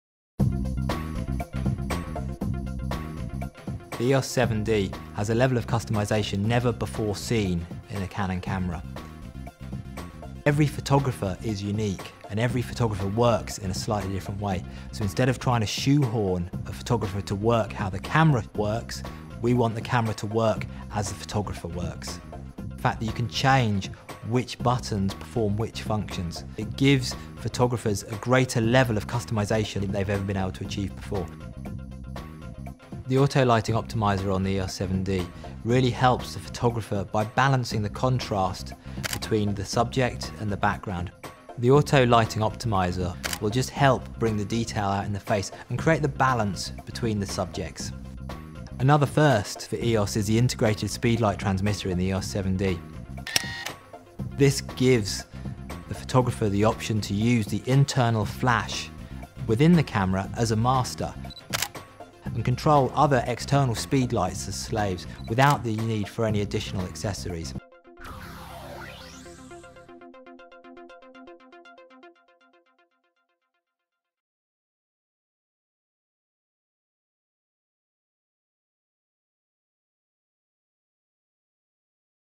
Canon EOS 7D - Your camera ... 5/6
Mike Owen - European Product Planning Manager - Photo Canon Europe introduces Canon's latest digital Pro SLR, and talks about the new Technologies, customs controls and more